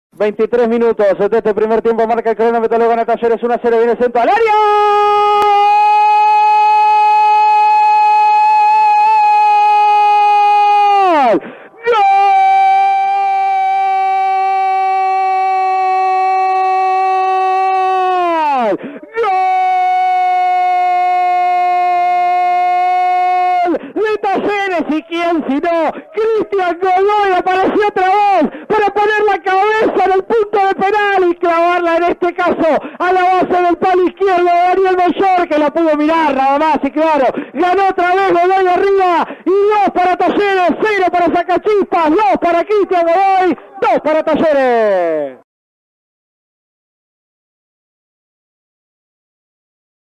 Gol Cristian Godoy 2 Fecha 7° 21 - Torneo Apertura - Talleres 4 - Sacachispas 0
Gol de Cristian Rodrigo Godoy.
Relato: Martín Buongiorno.